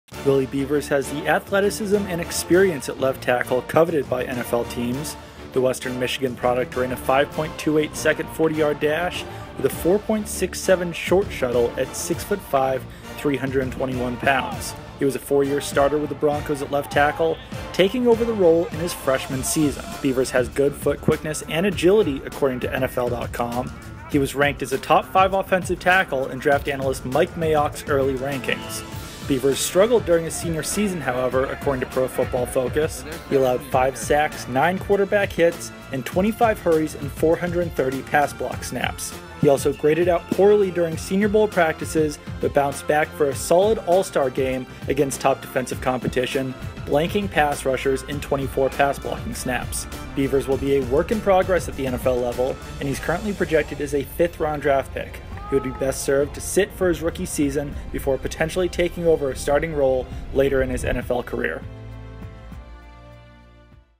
2016 NFL Draft Profile: Willie Beavers, OT, Western Michigan
NESN.com's Doug Kyed breaks down offensive tackle prospect Willie Beavers, a four-year starter out of Western Michigan.